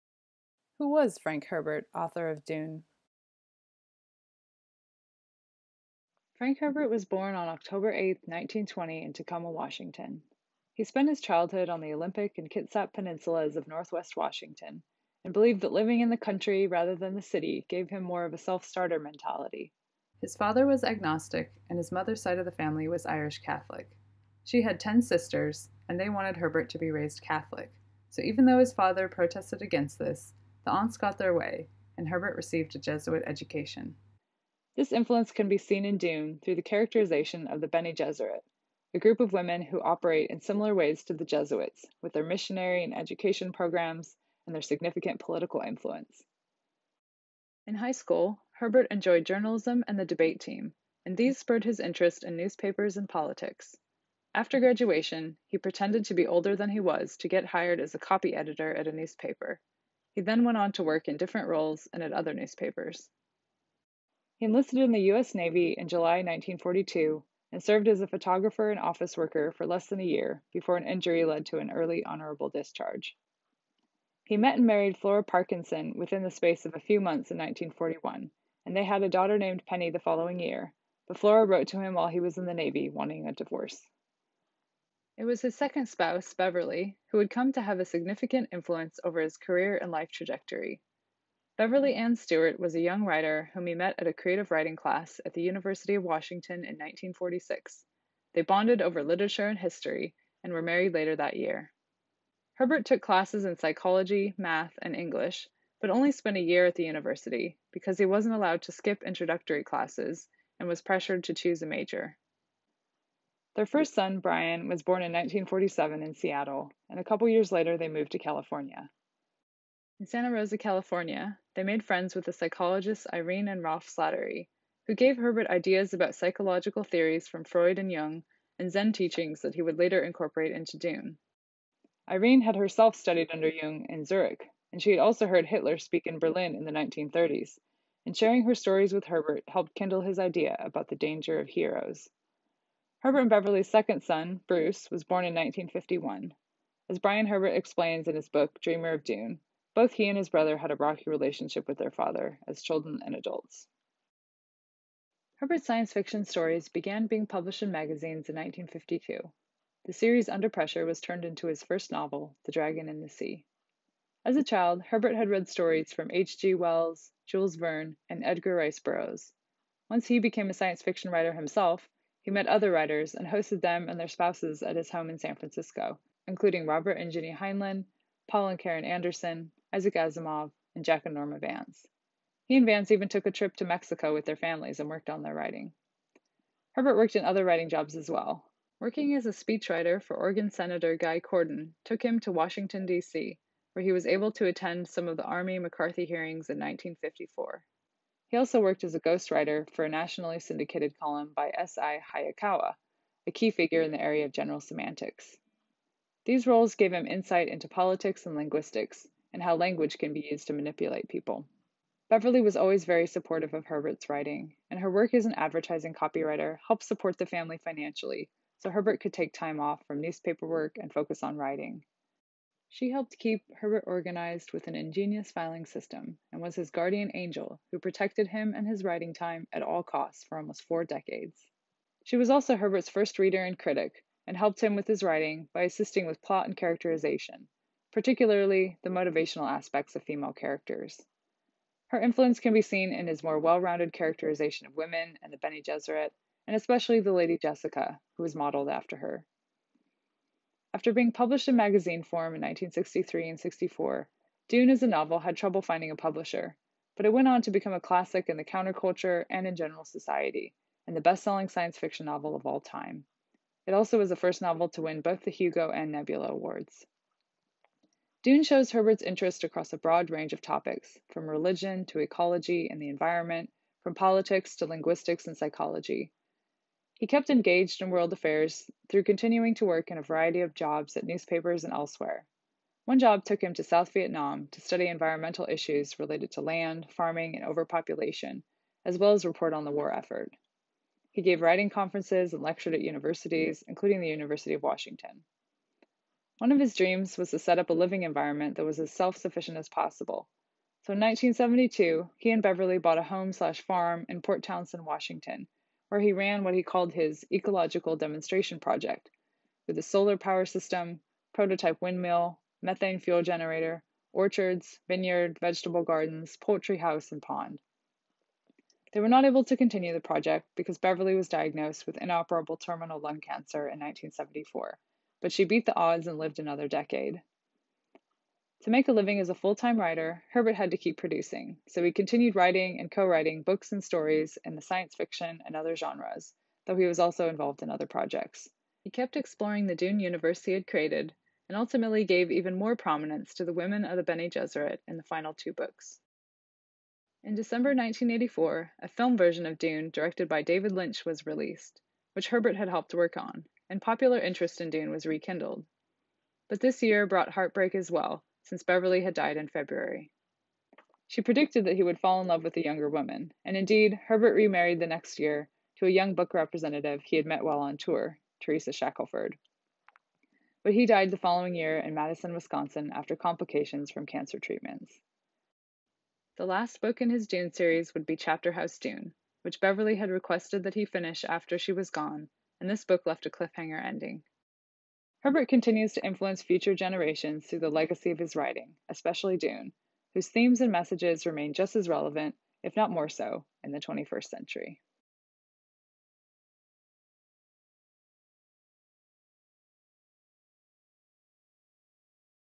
Who was Frank Herbert, author of Dune?
This select biography of author Frank Herbert offers highlights of his personal and professional life and the figures who influenced his writing, such as his Irish Catholic aunts, writing partner and spouse Beverly, and other science fiction writers.